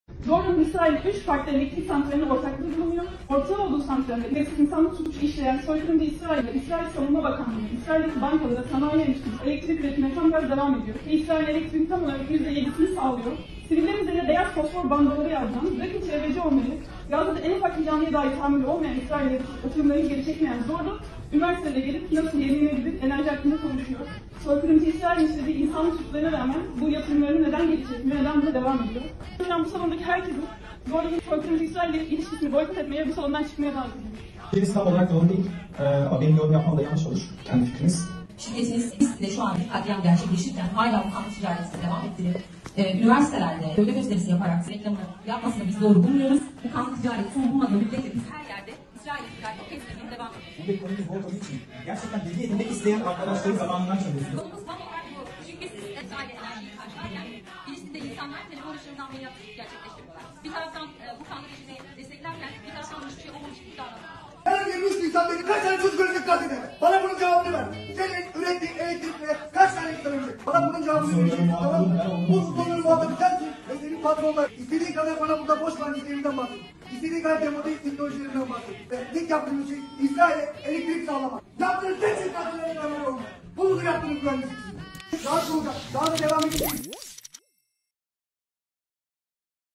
بينما يقتل الناس بغزة تتحدث عن الطاقة المتجددة!
طلاب جامعة بهتشه شهير التركية يقاطعون كلمة لمسؤول من "زورلو" القابضة لمطالبة الشركة بوقف استثماراتها مع الاحتـ،ـلال وقطع علاقاتها التجارية معه.

يذكر أن الشركة القابضة والتي تعمل في مجال الطاقة تمتلك استثمارات في الكيان الإسرائيلي تقدر بأكثر من مليار دولار، كما تمد مستوطنات الاحتـ،ـلال بالكهرباء.

وإن وددت متابعتنا على باقي منصاتنا، يمكنكم الضغط على الروابط أدناه لتكونوا ضمن عائلة “نون بوست” على مواقع التواصل الاجتماعي
فيسبوك: